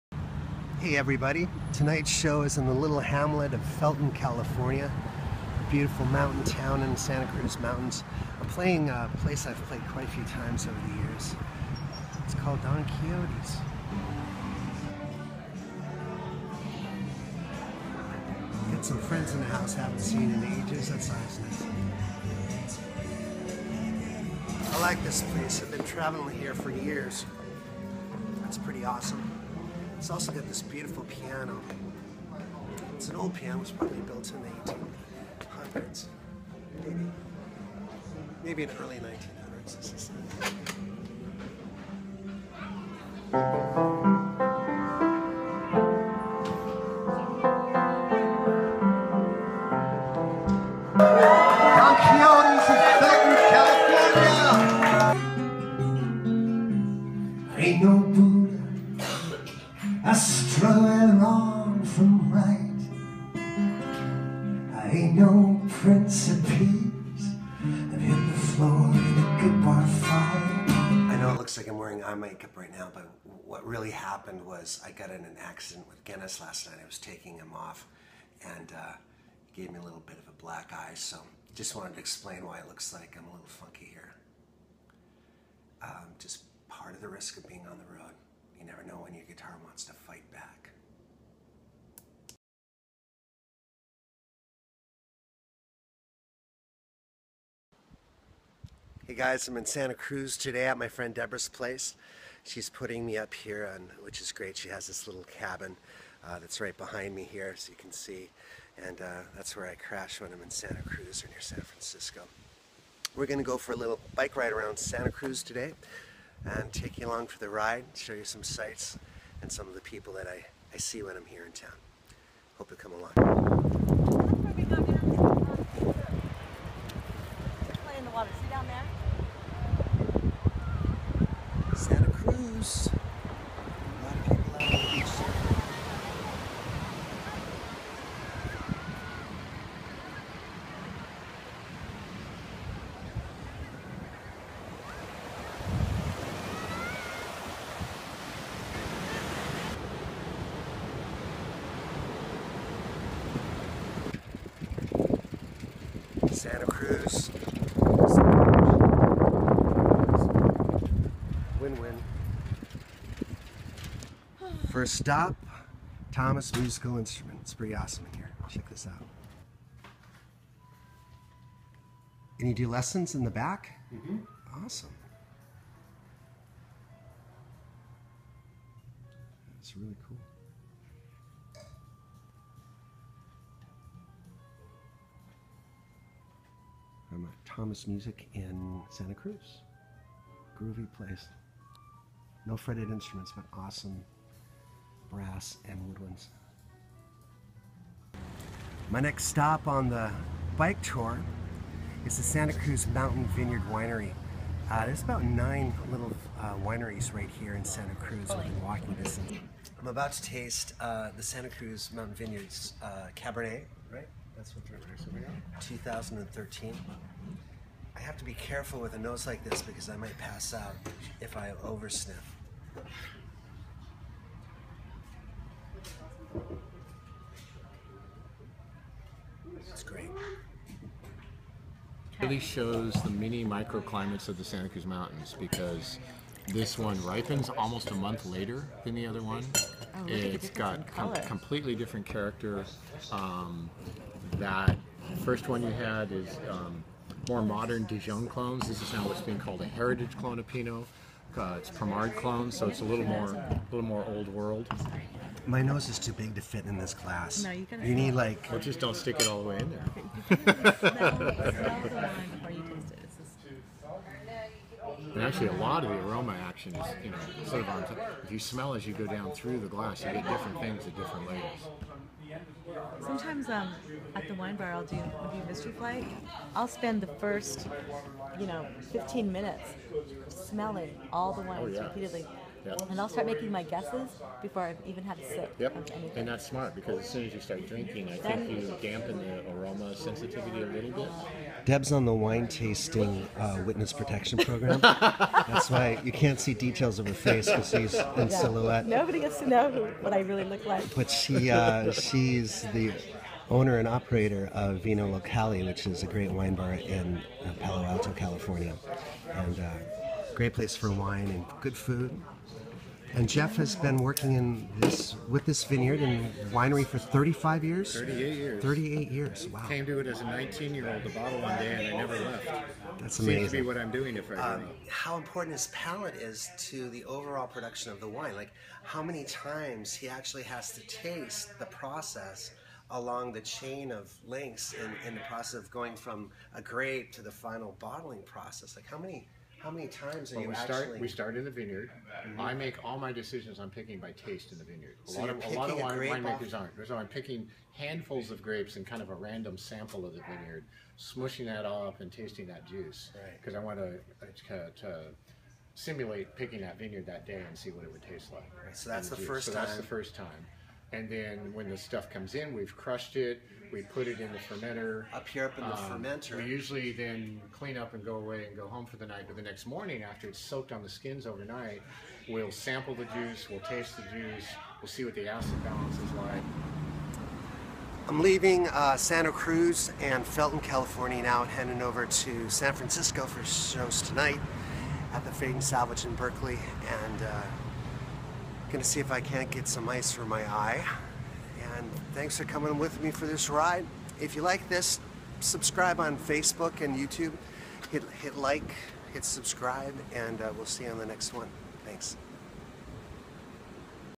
Ellis Paul: American Troubadour Vol. 8, Santa Cruz and Felton, CA
Ellis plays northern California, drinks wine, walks the ocean...